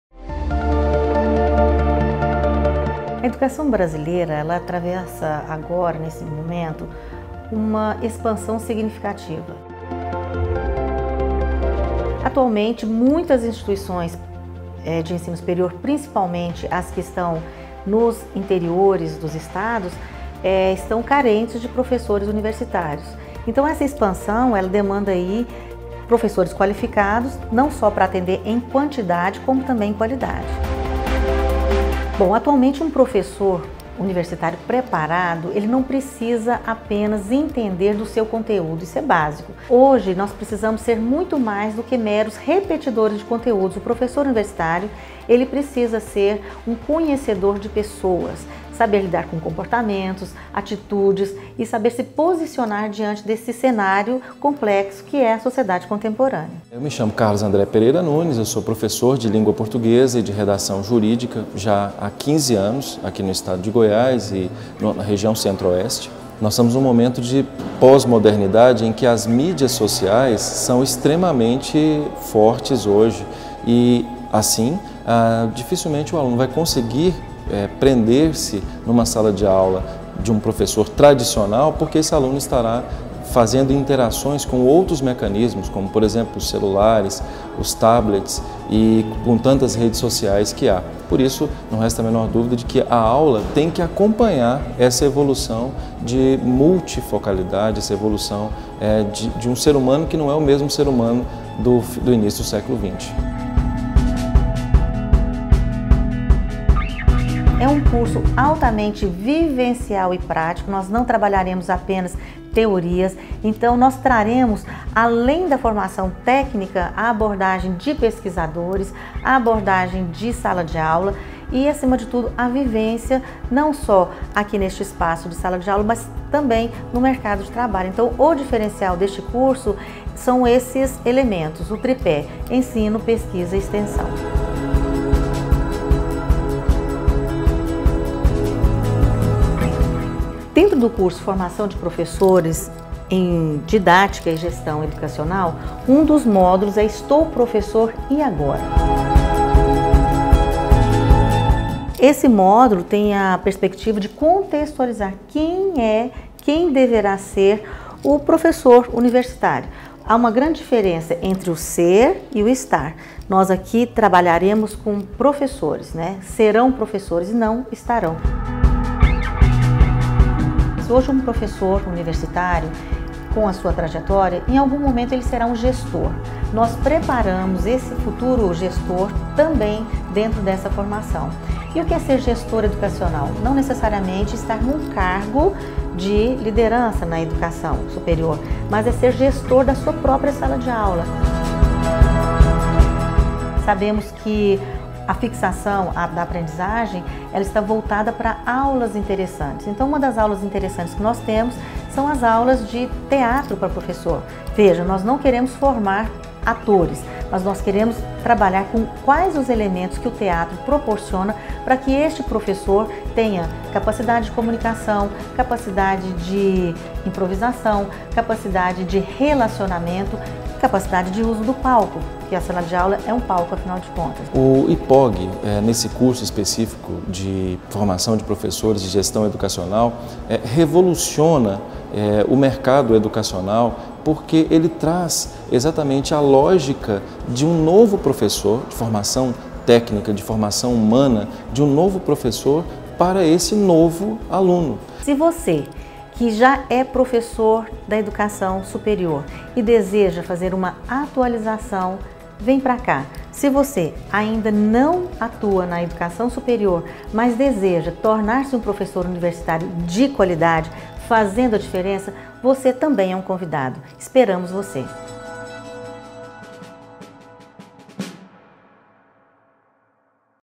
Especialização em Formação de Professores em Didática e Gestão Educacional
A atividade docente só pode ser exercida na medida em que a mesma seja objeto de qualificação por parte dos professores, que deixam de ser apenas mediadores do conhecimento para serem seus autores, seus gestores, ou seja, tendo-se uma visão e ações focadas na atividade profissional ativa, envolvendo todos os sujeitos da educação.

Conheça a especialização Formação de Professores em Didática e Gestão Educacional!